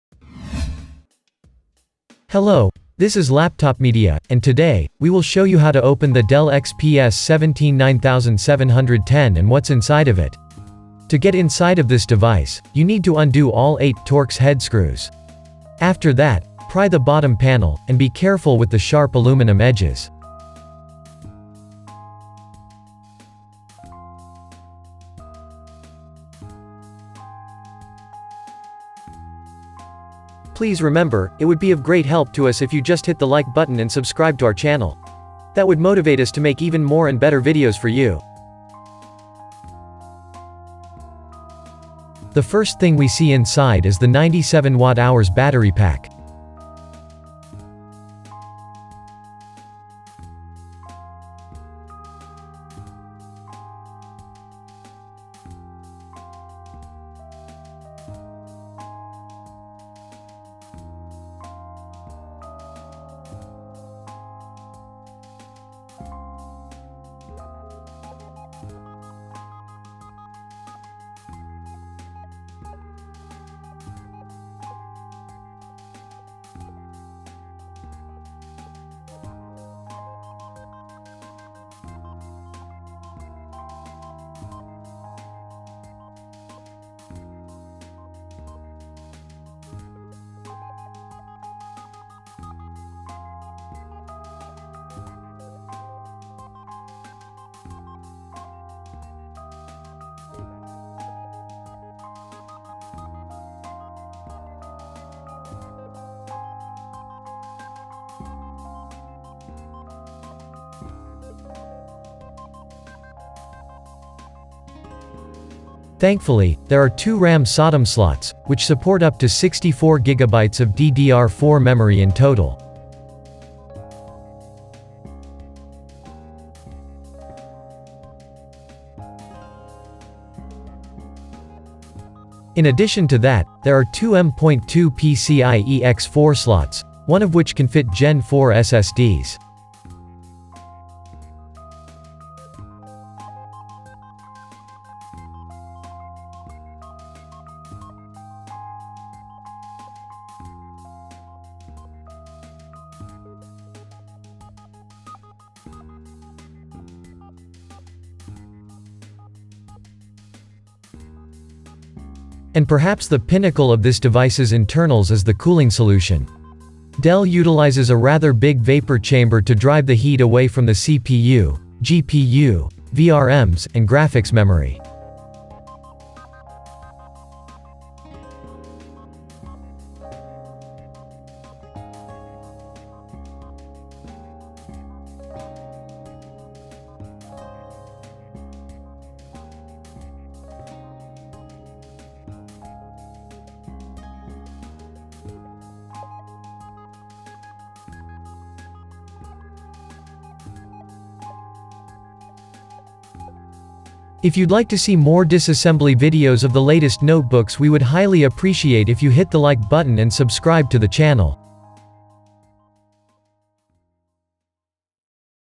🛠️ Dell XPS 17 9710 - disassembly and upgrade options
✅  LaptopMedia.com